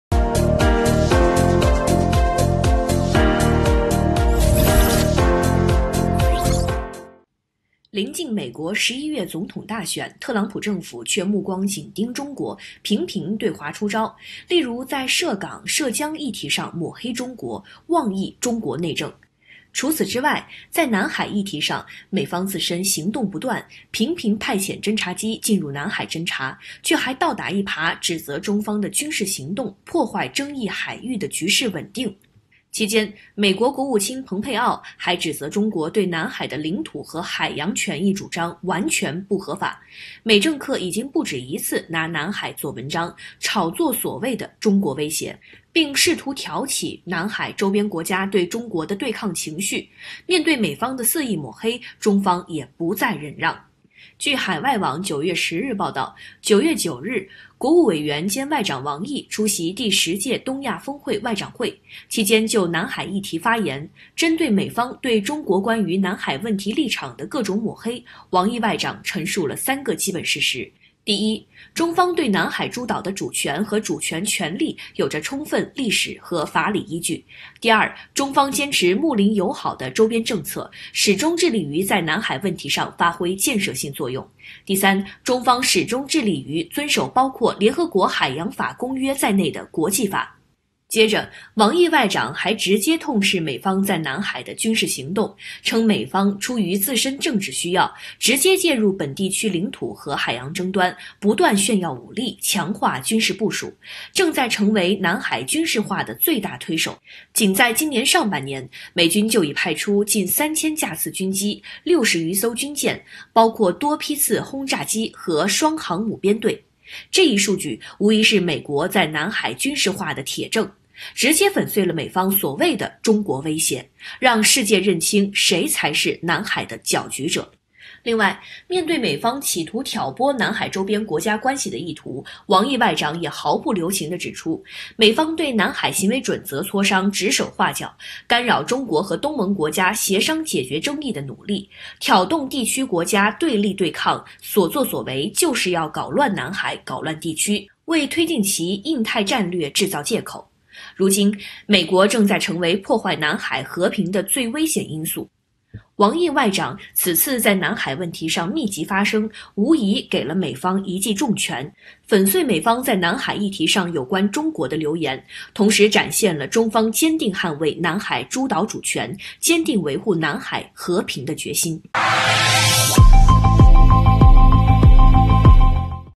还敢在南海搅局？一天内，王毅外长密集发声，列铁证给美一记重拳
还敢在南海搅局？一天内，王毅外长密集发声，列铁证给美一记重拳,本视频由深度解局原创提供,434次播放,好看视频是由百度团队打造的集内涵和颜值于一身的专业短视频聚合平台